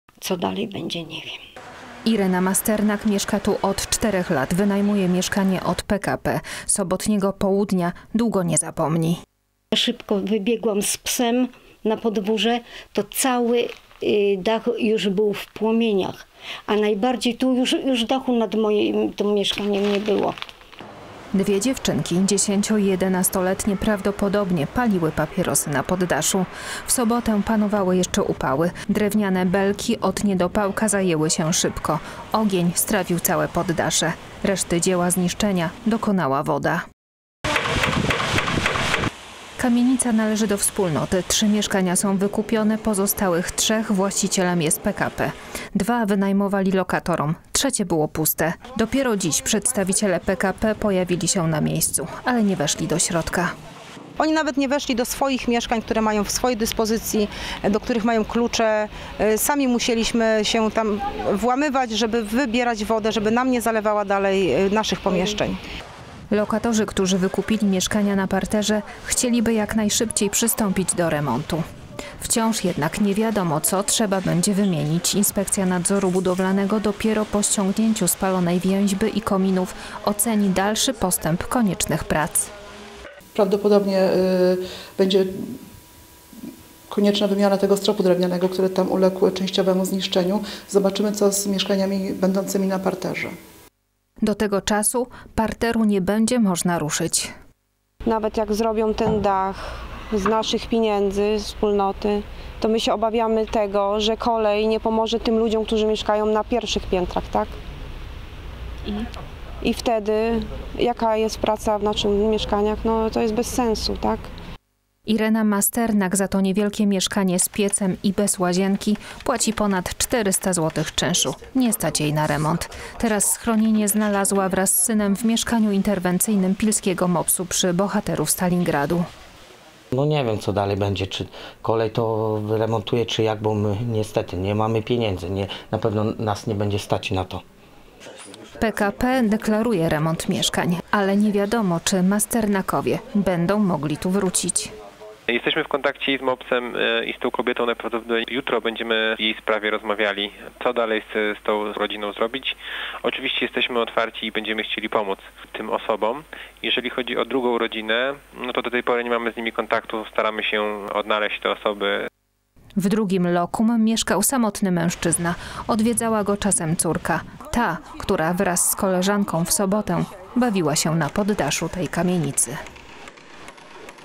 Piła: pogorzelcy chcą wrócić do swoich mieszkań
W sobotę spłonęła kamienica przy ul. Tczewskiej w Pile. Pogorzelcy chcą jak najszybciej wrócić do swoich mieszkań